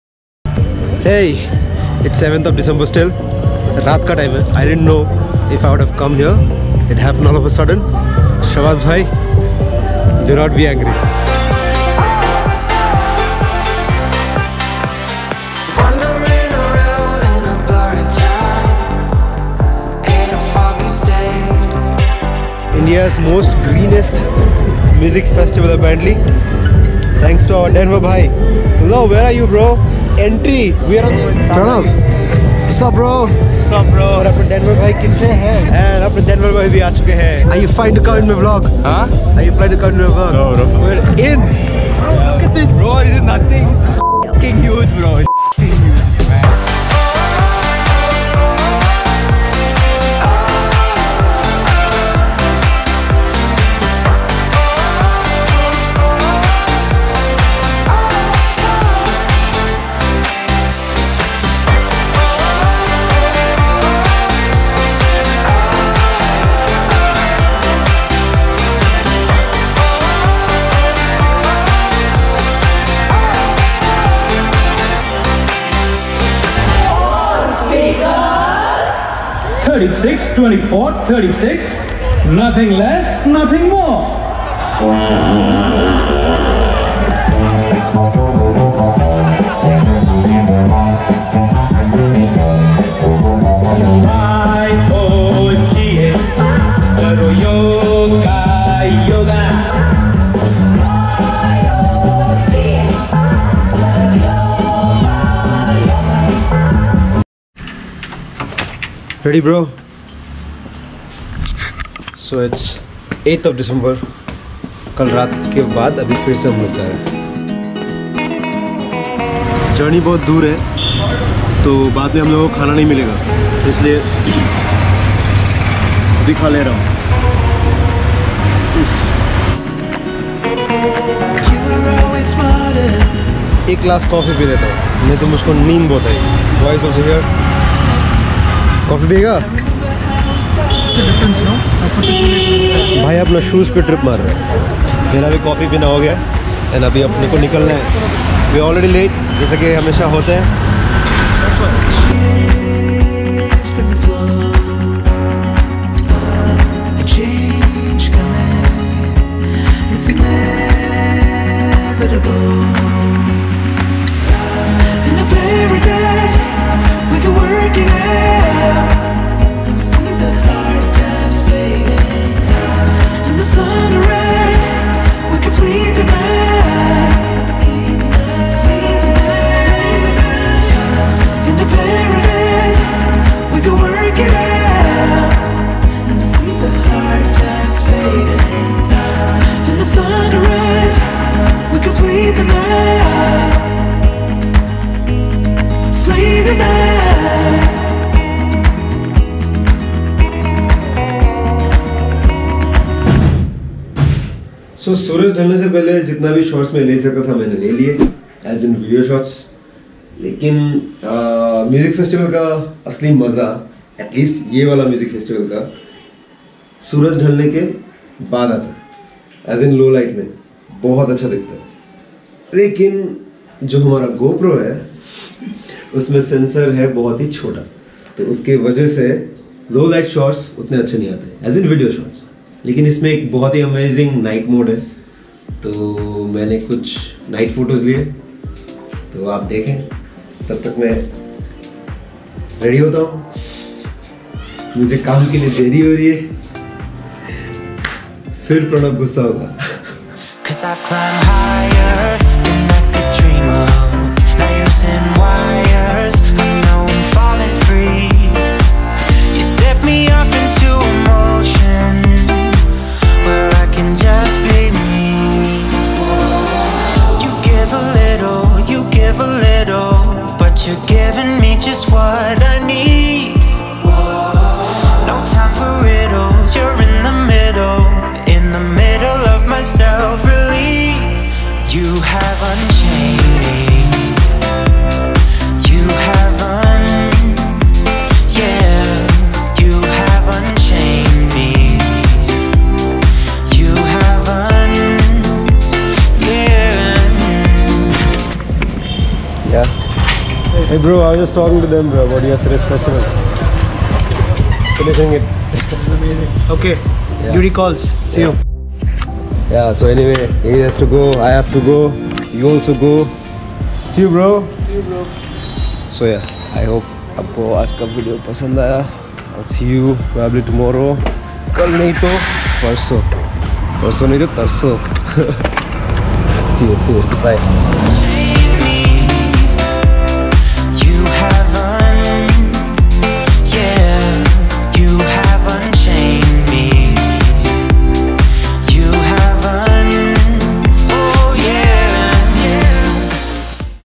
ECHOES OF EARTH 2019 MUSIC FESTIVAL ( Vlog)
Hello everyone, so I got to visit this amazing music festival called Echoes of the Earth that happened on the 7th and 8th of December 2019.
This video is all about the unplanned weekend that inspired me and my friend Pranav in the best way possible. All thanks to our boy Danver.
Also this is Episode 04 of Vibes.
Do check out the entire video and do share your views in the comment  section below.
Cheers :)

Follow Pranav and Danver on Instagram